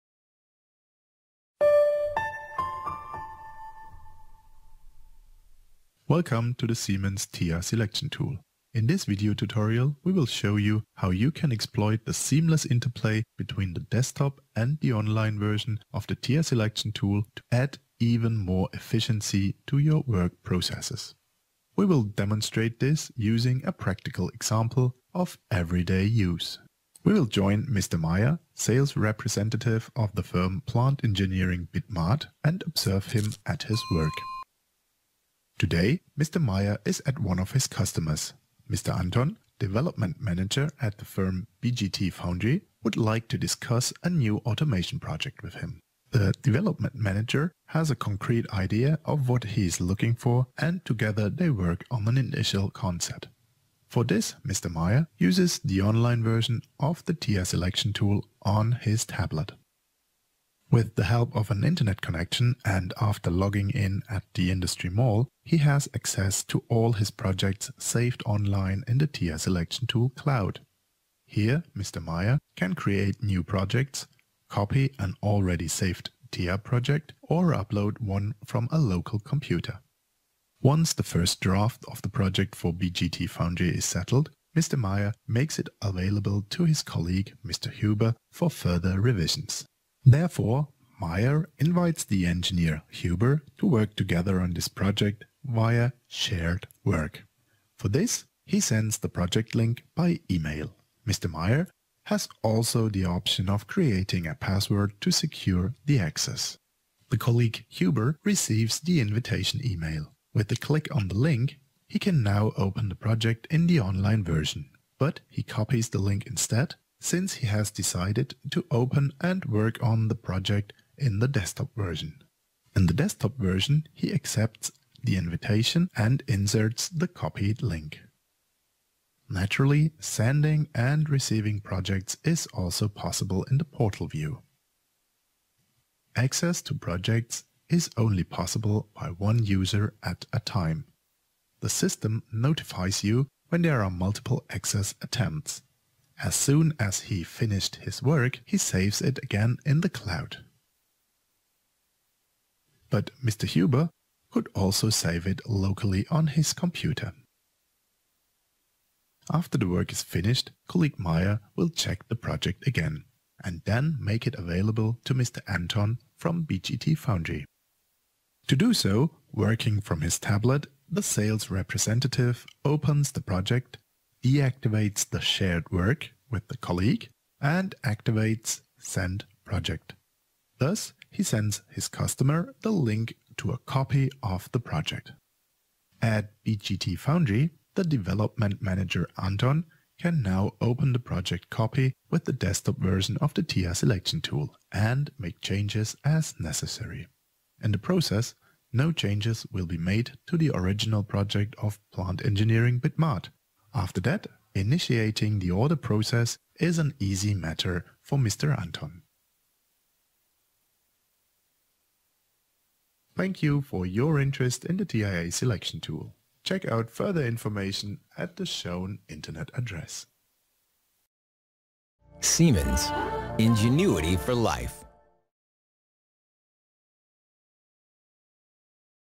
TIA Selection Tool - Interplay of the desktop and the online version
In this video tutorial we will show you how you can exploit the seemless interplay between the desktop and the online version of the TIA Selection Tool to add even more efficency to your work processes.